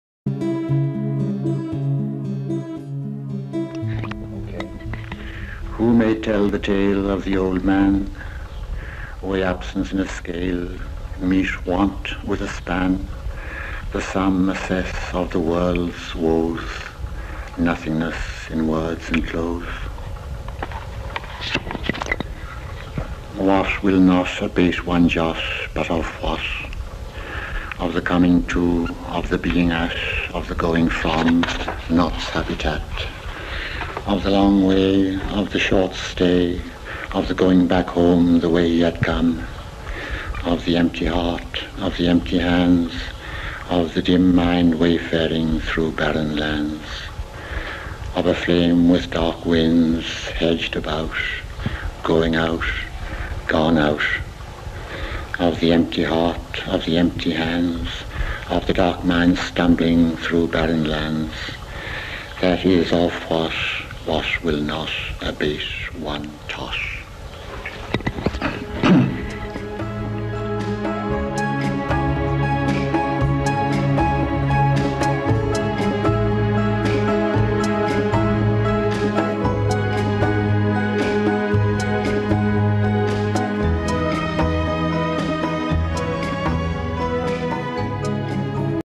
Samuel Beckett reads from Watt
The recordings were made in 1965 by Lawrence Harvey, professor of comparative literature at Dartmouth College, who traveled to Paris to meet with Beckett a number of times from 1961 to 1965 while researching his 1970 book Samuel Beckett, Poet and Critic. (Poetry Foundation)